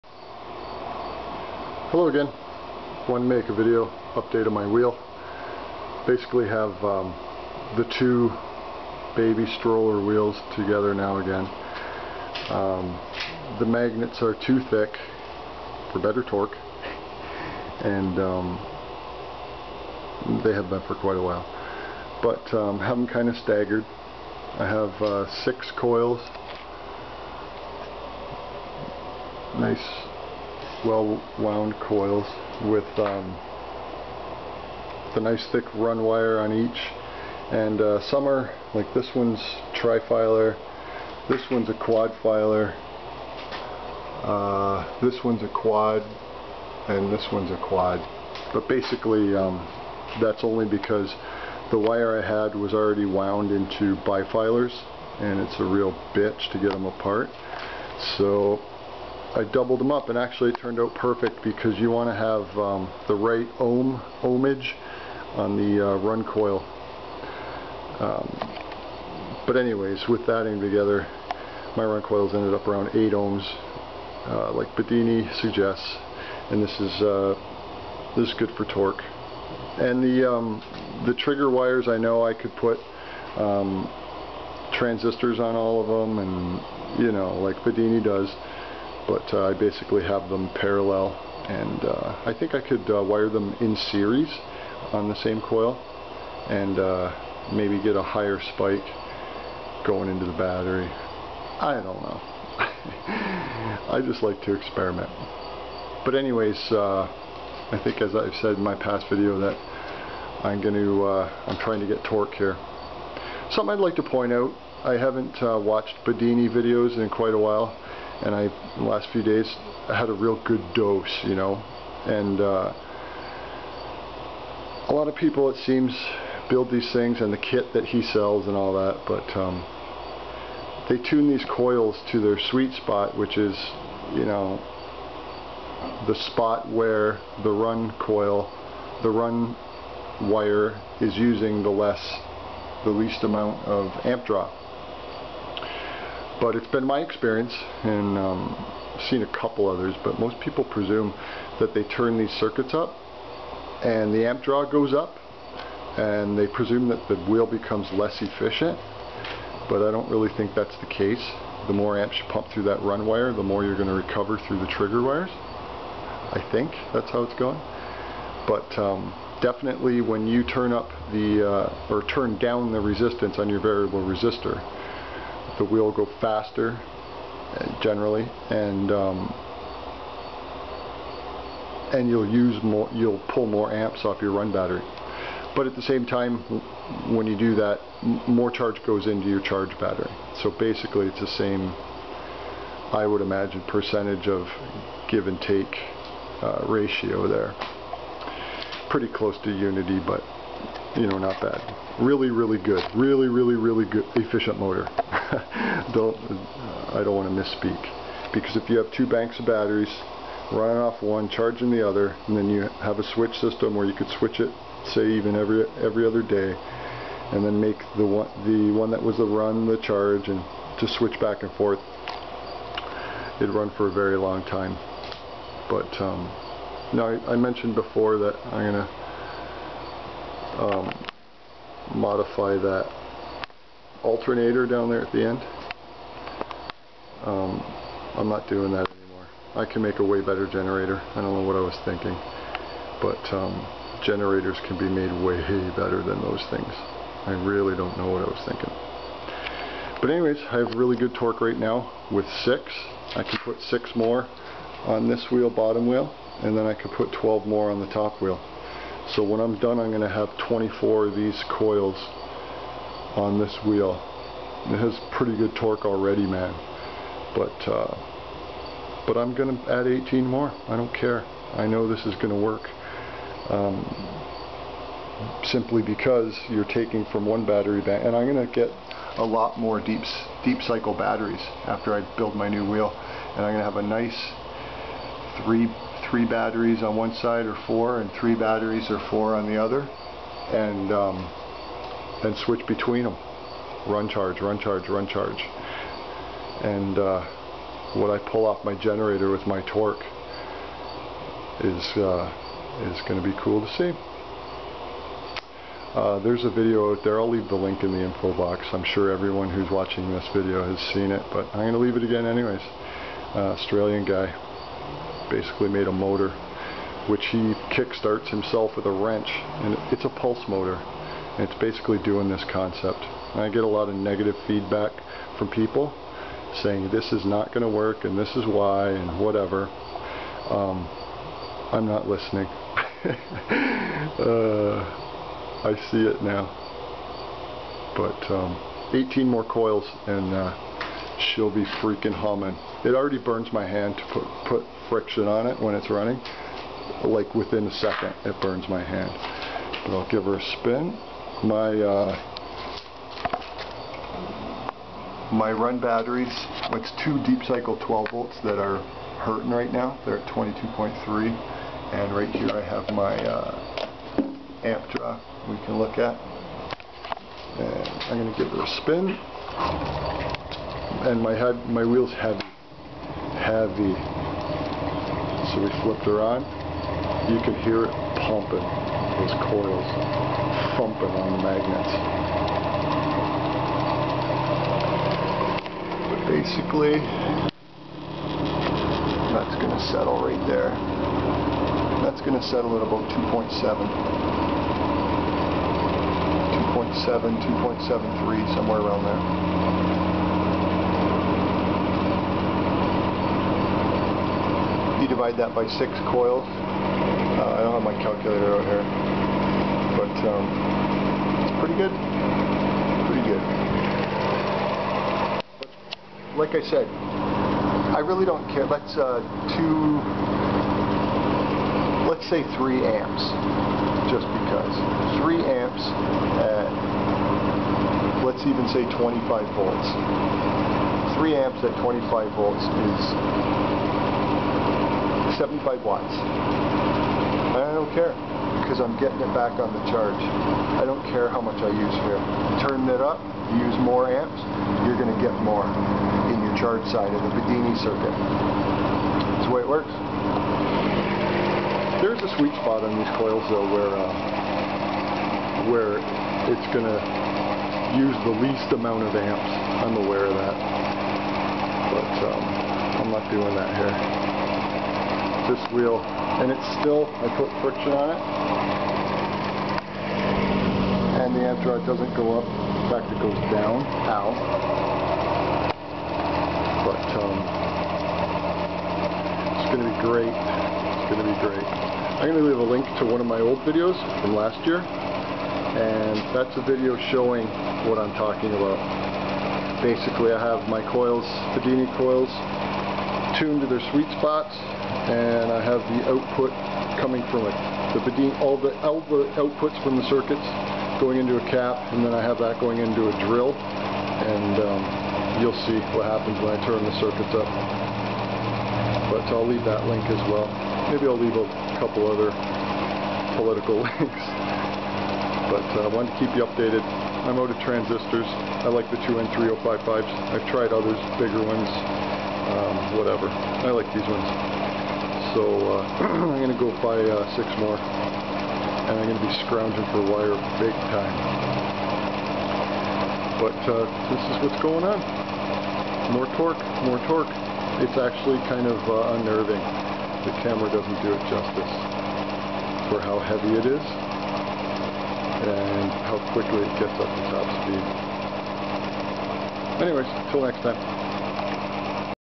Bedini Energizer - 6 Coiler
18 more coils to go! I have to build a new wheel very soon because the cheapo bearrings from the baby stroller wheels are brutal...and this wheel is becoming a beast already. In the end I guess it will suck 4x as much energy as it does now but it will send most of it back to the charge side. With 6 coils it draws 2.7amps (0.45amps/coil) at 21.5 or so volts 2.7x21.5= 58.08 watts. 58.08x4=232.2 watts estimated total for 24 coils.

I will probably use thin gauge HV wire for the generator coils...we'll see...but the more ohms on your generator coils will result in less drag on the wheel...I've noticed this from experimenting in the past year. A Kromrey style seems to be the best for generation. Here is a link to the pulse motor the guy in Australia made:

Here is a link to my video from last year that shows Bedini output at near 0 resistance/coil compared to tuned to the coil's "sweet spot:"